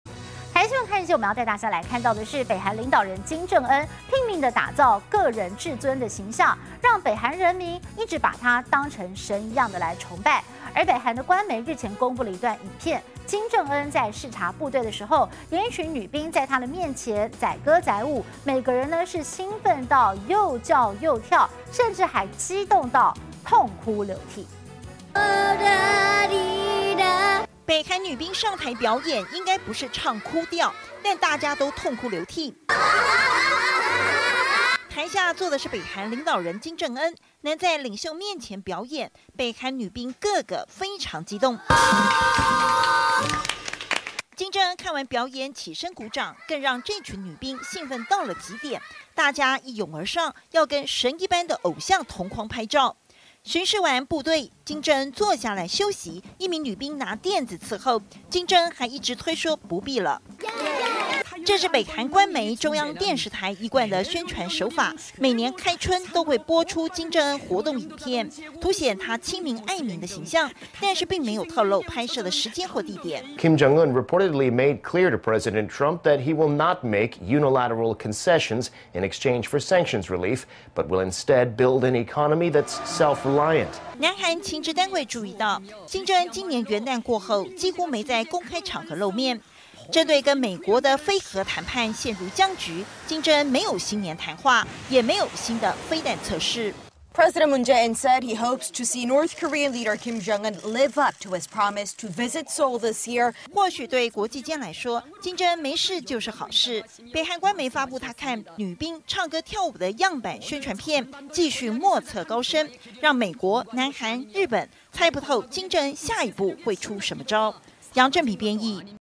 北韓官媒宣傳手法 展現金正恩親民形象
北韓領導人金正恩拼命打造「個人至尊」的形象，讓北韓人民一直把他當作神來崇拜。北韓官媒日前公布一段影片，金正恩視察部隊，一群女兵在他面前表演載歌載舞，每個人興奮到又叫又跳，甚至激動到痛哭流涕，非常誇張。